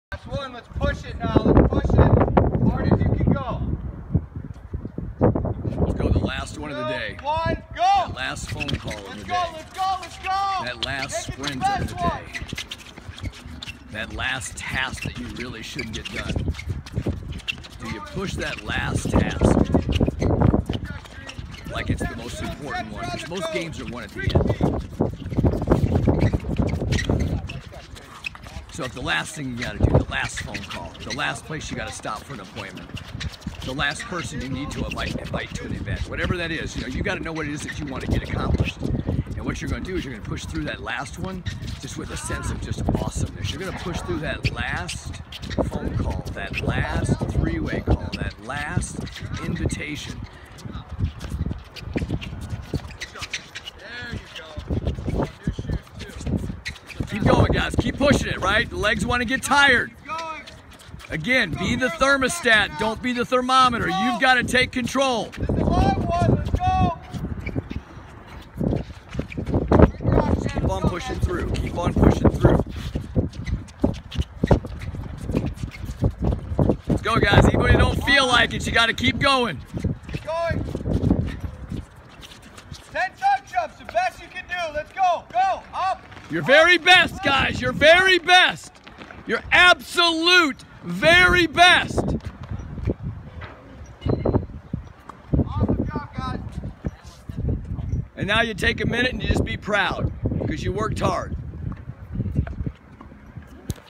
Push hard at the end to achieve all that you desire!
What we lacked in Temperature today  we made up for in hard work. 5 hours into our workout today we finished strong!